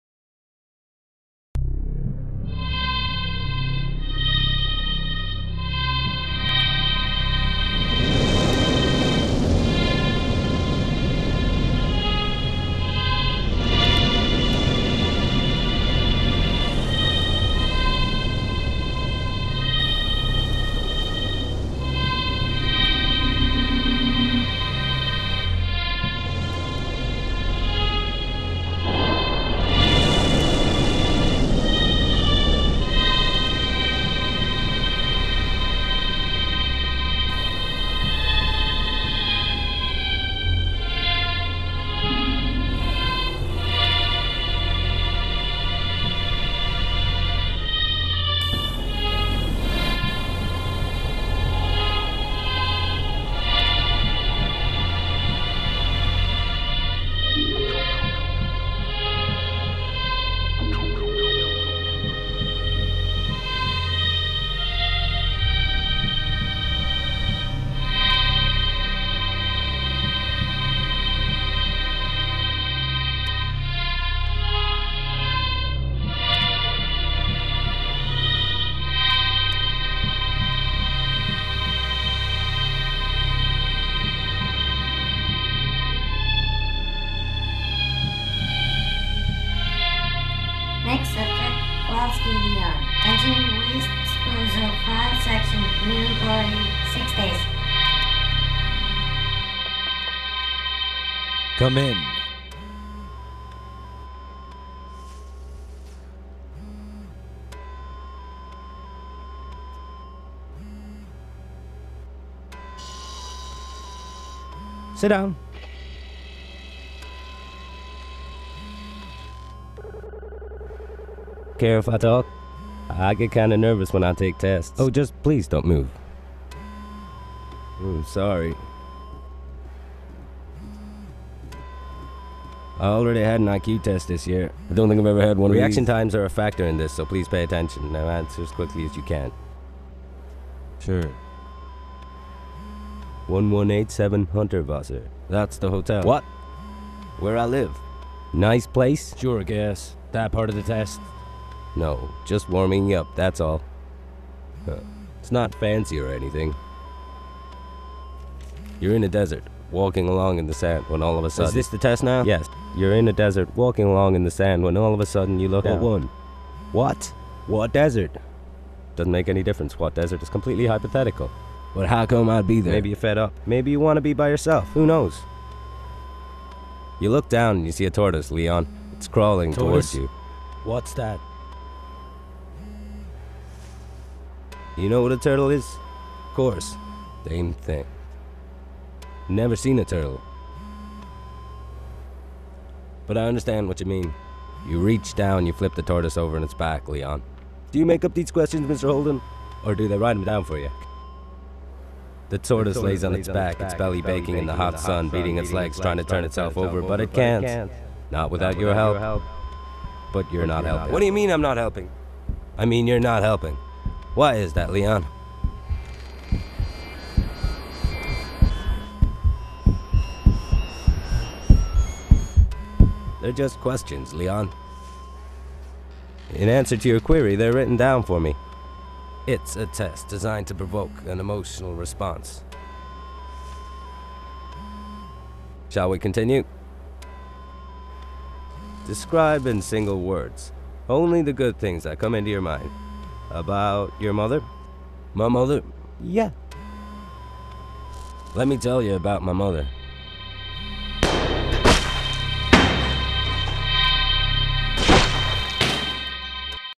Peter O'Malley Blade Runner Scene
Audio replacement for intro scene to Blade Runner. All music original